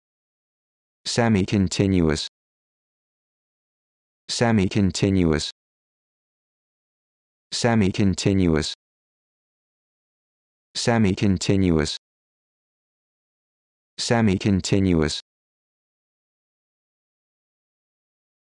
How to Pronounce Semicontinuous
This video shows you how to pronounce Semicontinuous